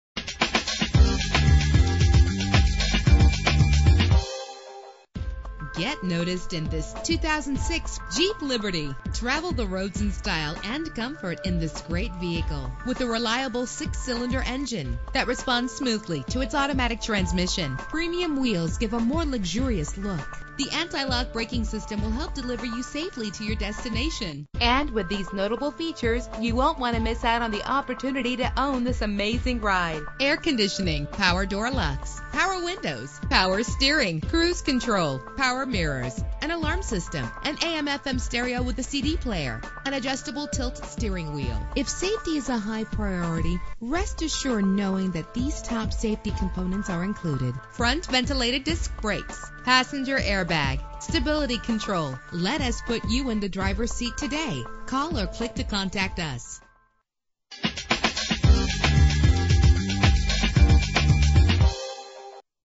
2006 Jeep Liberty - Daphne AL
Year: 2006
Make: Jeep
Model: Liberty
Trim: Limited
Engine: 3.7 liter 6 cylinder 12 valve
Transmission: Automatic
Color: Deep Beryl Green Pearl
Mileage: 81536
Address:
27161 Hwy 98